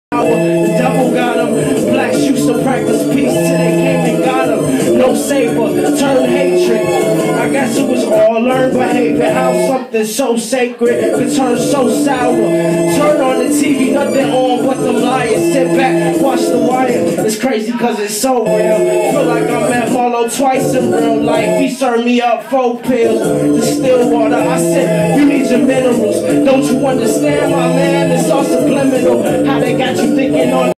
Jeremiah Jae LIVE HOUSE SHOW in San Diego - 01/18/2012
Kill Quanti hosting Brainfeeder's Jeremiah Jae for an intimate family room live set.

Trust us. We will not hurt u.